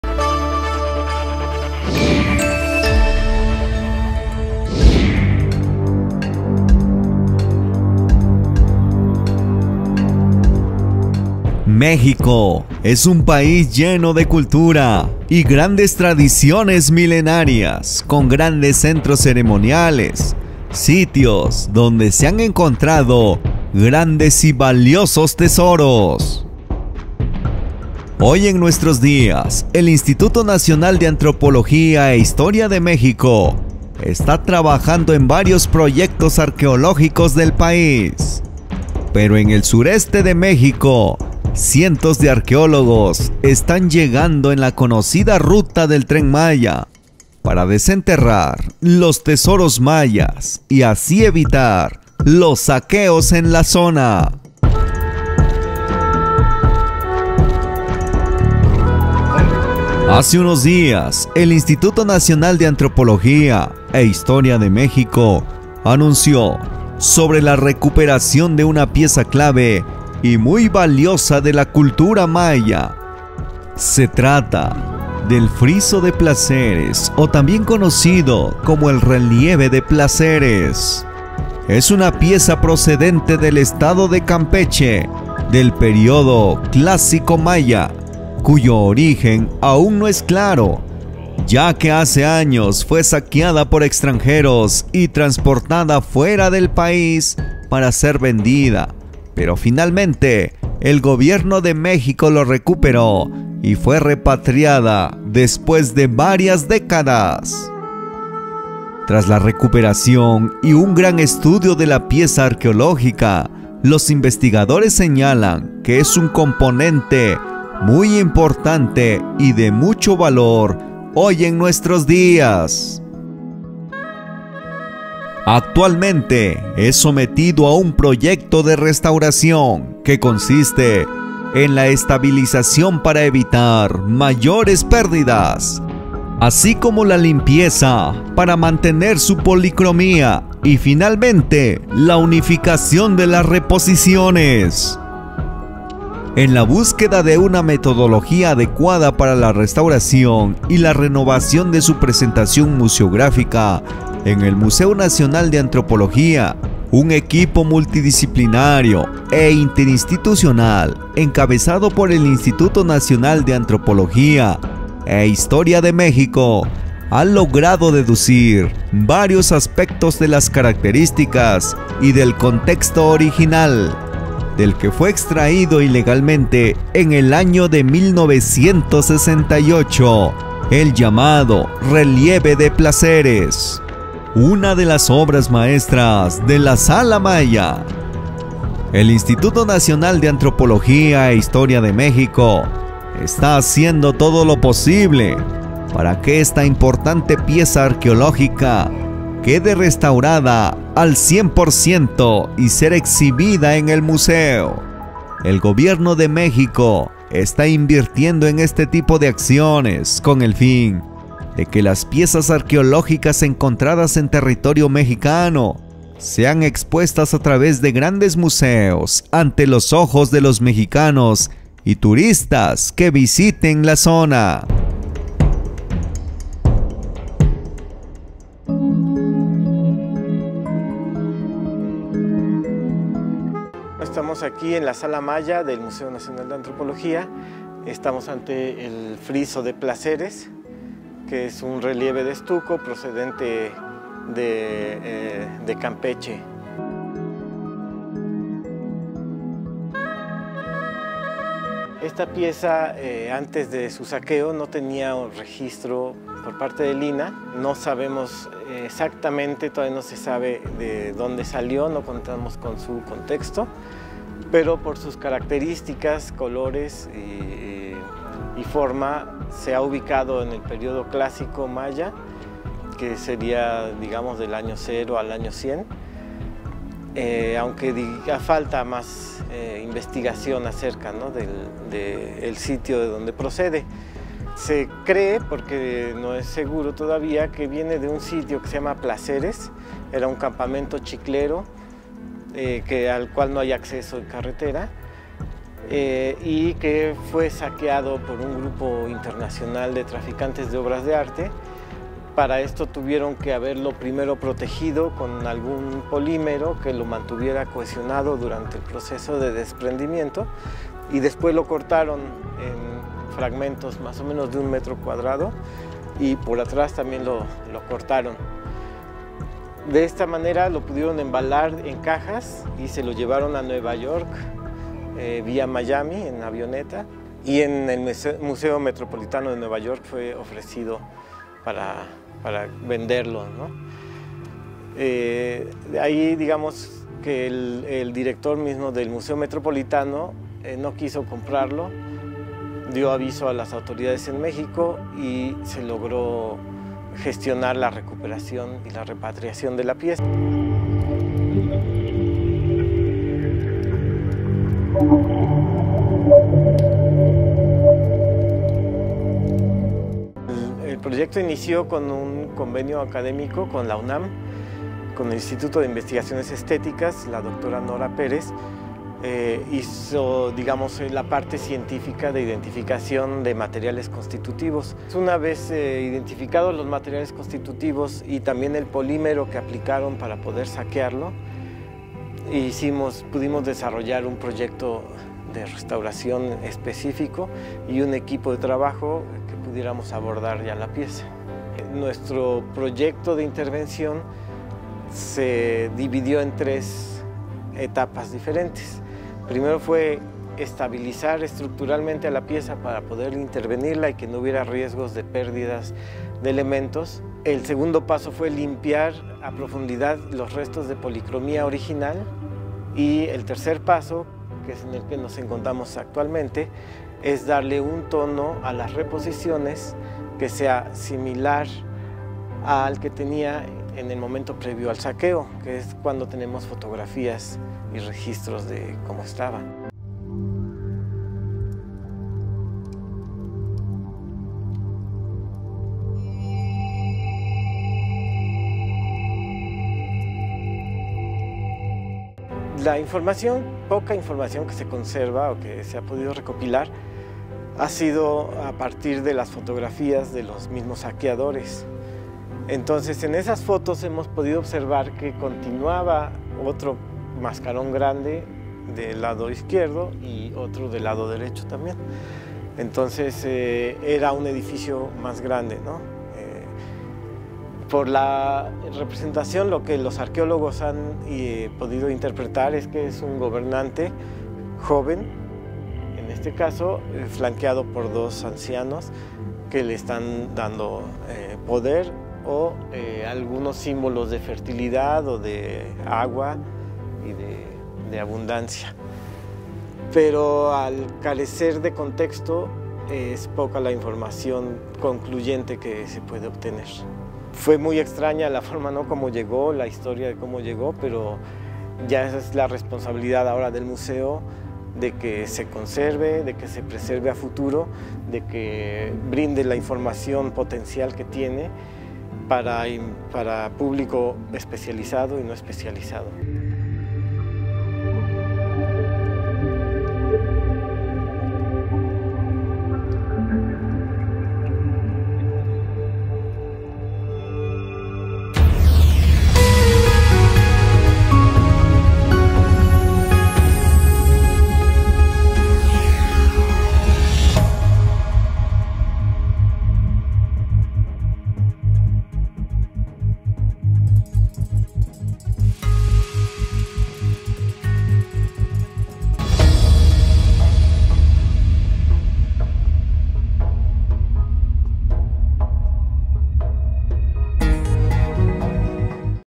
😮Mira! así es la sorprendente pieza MAYA que rescató México de mano de los saqueadores extranjeros
Saqueadores extranjeros se llevan pieza de gran valor, Mexico logra rescatar El "Relieve de Placeres", un friso maya que se encuentra en el Museo Nacional de Antropología (MNA) se encuentra casi listo para ser exhibido.Mira!